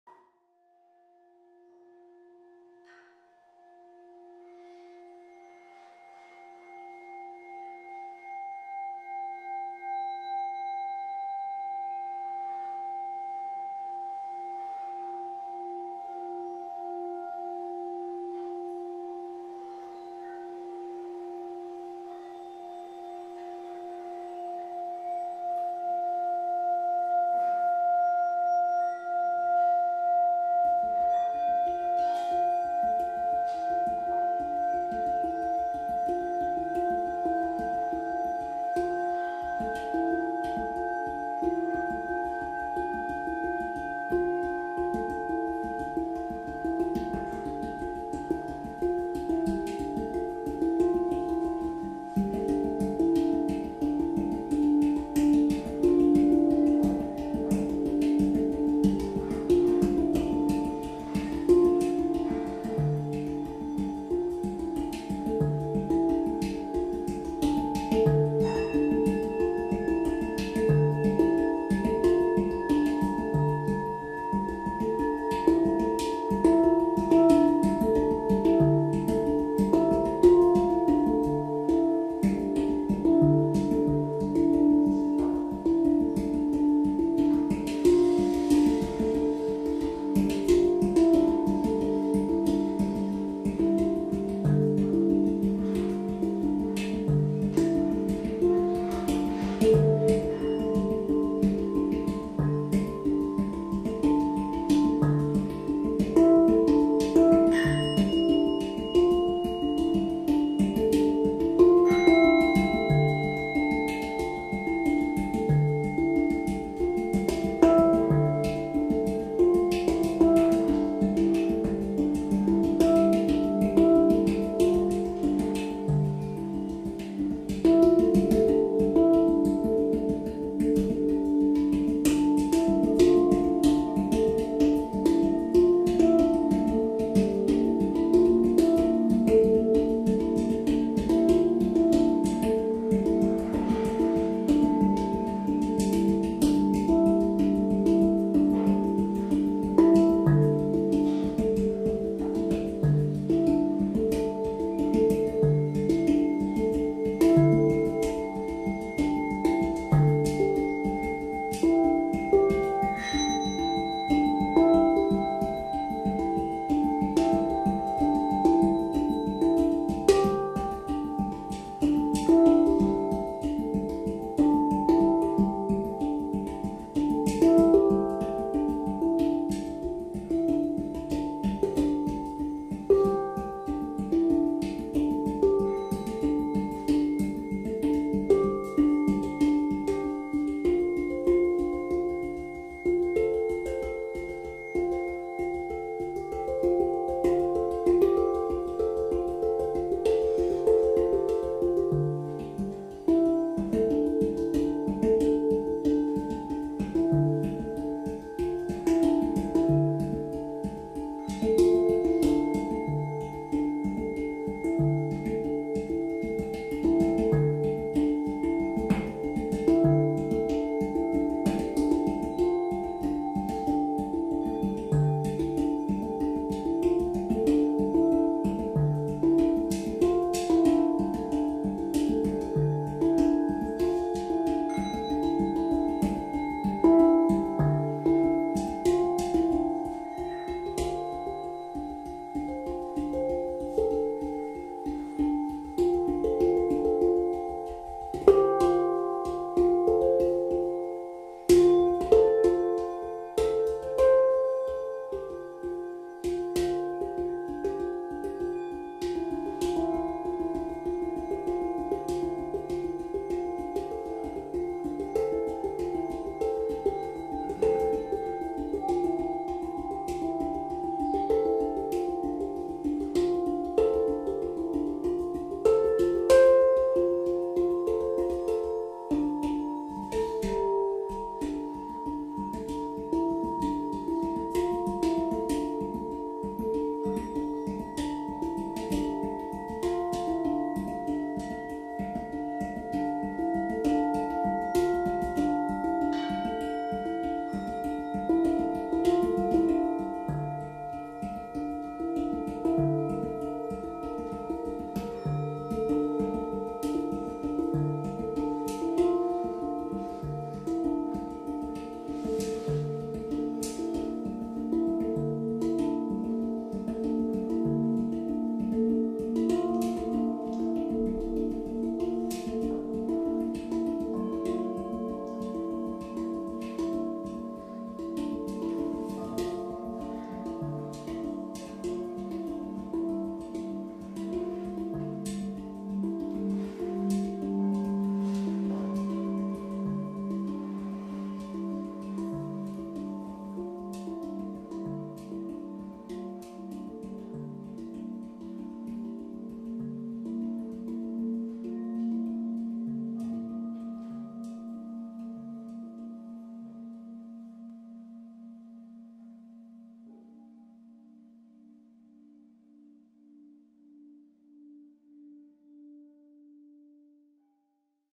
Friedhelm Schöck: Klangschalen und Handpan 2 (Singing Bowls and Handpan 2)
Handpan aufgenommen am 8.5.2017
Klangschalen aufgenommen während eines Konzerts im Kerner in Marburg am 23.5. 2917
Handpan recorded in Munich
Singing Bowls recorded during a Concert in Marburg.